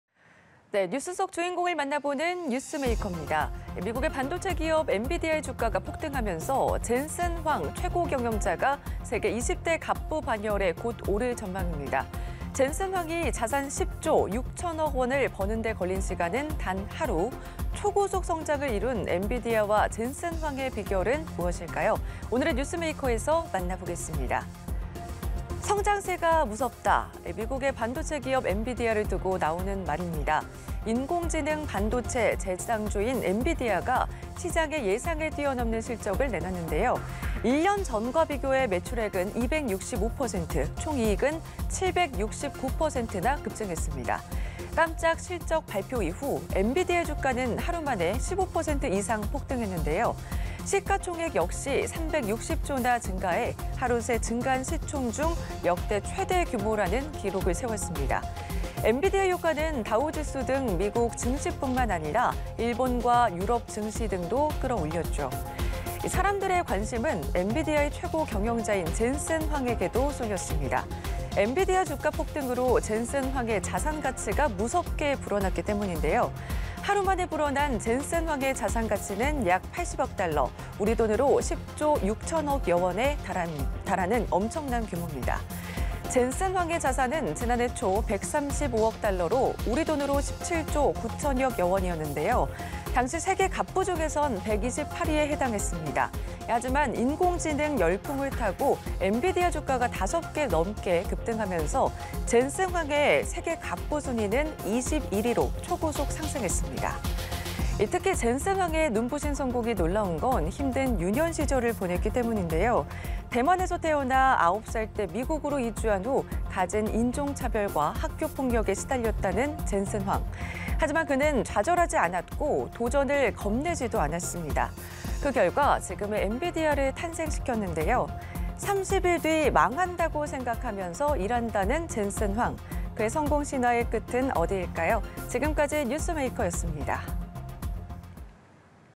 [뉴스메이커] 엔비디아 주가 급등에…CEO 젠슨 황, '세계 20대 갑부' 진입 눈앞 / 연합뉴스TV (YonhapnewsTV)
[뉴스메이커] 엔비디아 주가 급등에…CEO 젠슨 황, '세계 20대 갑부' 진입 눈앞

뉴스 속 주인공을 만나 보는 '뉴스메이커'입니다.

미국의 반도체 기업 엔비디아의 주가가 폭등하면서 젠슨 황 최고경영자가 세계 20대 갑부 반열에 곧 오를 전망입니다.

젠슨 황이 자산 10조 6000억 원을 버는 데 걸린 시간은 단 하루.

초고속 성장을 이룬 엔비디아와 젠슨 황의 비결은 무엇일까요?

오늘의 뉴스메이커에서 만나 봅니다.

"성장세가 무섭다." 미국의 반도체 기업 엔비디아를 두고 나오는 말입니다.

인공지능 반도체 대장주인 엔비디아가 시장의 예상을 뛰어넘는 실적을 내놨는데요.

1년 전과 비교해 매출액은 265%, 총이익은 769%나 급증했습니다.

깜짝 실적 발표 이후, 엔비디아 주가는 하루 만에 15% 이상 폭등했는데요.

시가총액 역시 360조나 증가해 하루 새 증가한 시총 중 역대 최대 규모라는 기록을 세웠습니다.

엔비디아 효과는 다우지수 등 미국 증시뿐만 아니라 일본과 유럽 증시 등도 끌어올렸죠.

사람들의 관심은 엔비디아의 최고경영자인 젠슨 황에게도 쏠렸습니다.

엔비디아 주가 폭등으로 젠슨 황의 자산 가치가 무섭게 불어났기 때문인데요.

하루 만에 불어난 젠슨 황의 자산 가치는 약 80억 달러, 우리 돈으로 10조 6천여억 원에 달하는 엄청난 규모입니다.

젠슨 황의 자산은 지난해 초 135억 달러로 우리 돈으로 17조 9천여억원이었는데요.

당시 세계 갑부 중에선 128위에 해당했습니다.

하지만 인공지능 열풍을 타고 엔비디아 주가가 5배 넘게 급등하면서, 젠슨 황의 세계 갑부 순위는 21위로 초고속 상승했습니다.

특히 젠슨 황의 눈부신 성공이 놀라운 건 힘든 유년 시절을 보냈기 때문입니다.

대만에서 태어나 9살 때 미국으로 이주한 후 갖은 인종차별과 학교 폭력에 시달렸다는 젠슨 황.

하지만 그는 좌절하지 않았고 도전을 겁내지도 않았습니다.

그 결과 지금의 엔비디아를 탄생시켰는데요.

"30일 뒤 망한다고 생각하면서 일한다"는 젠슨 황.

그의 성공 신화의 끝은 어디일까요.

지금까지 '뉴스메이커'였습니다.